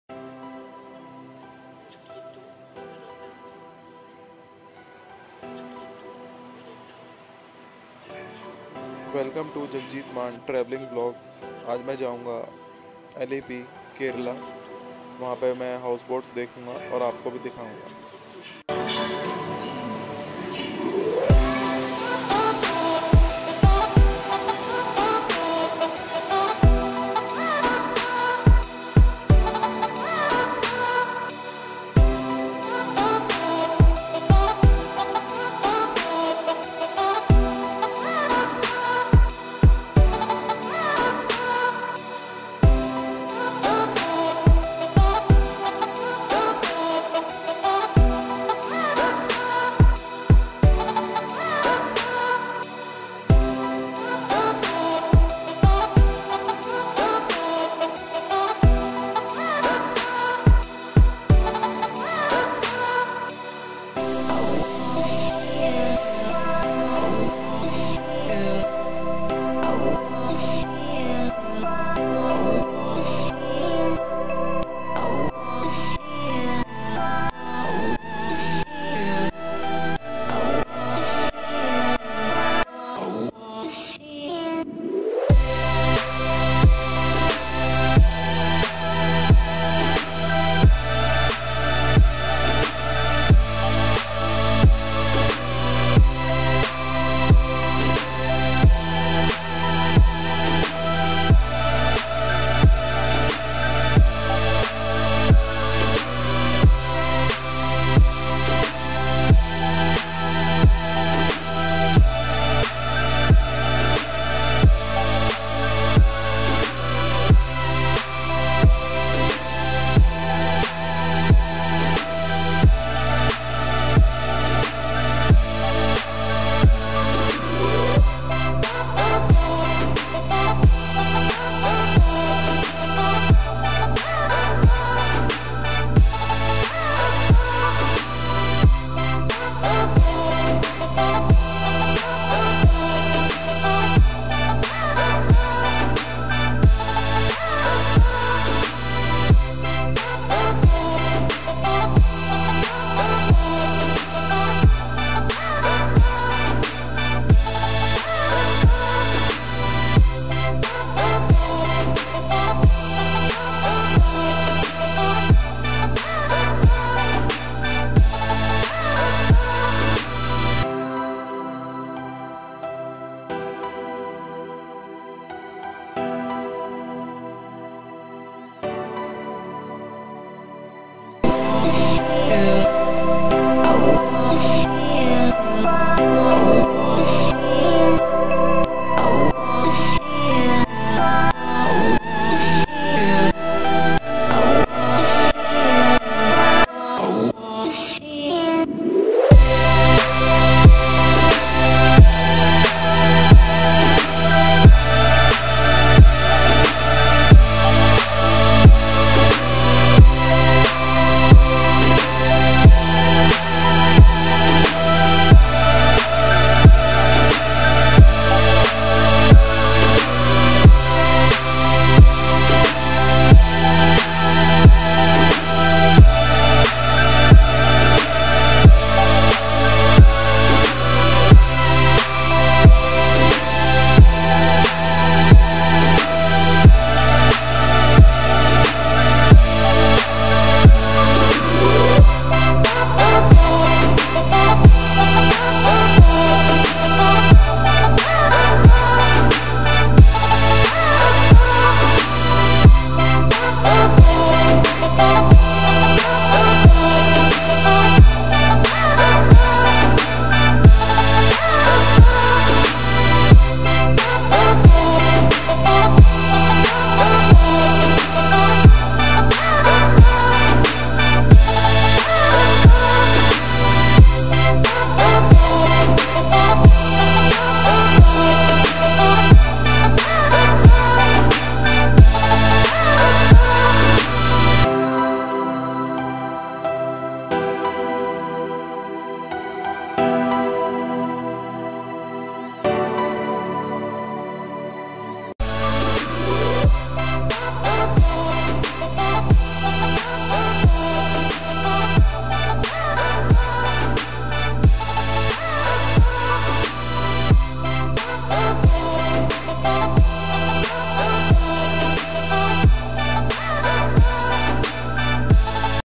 Alleppey Houseboat Trip Experience | Kerala Allappuzha Backwaters Houseboat| Lifestlye of Alleppey
Kerala Backwaters - what can I say about them. You have to experience them at least once in your life! And a houseboat ride is one of the must do things in Kerala. Hope you enjoyed my Houseboat tour video from Alleppey Kerala
Alleppey - new name - Alappuzha  is a very famous tourist destination in Kerala, known largely for backwaters houseboat experience and sea food.  We spent one full day at Alleppey explored a lot of things in one day.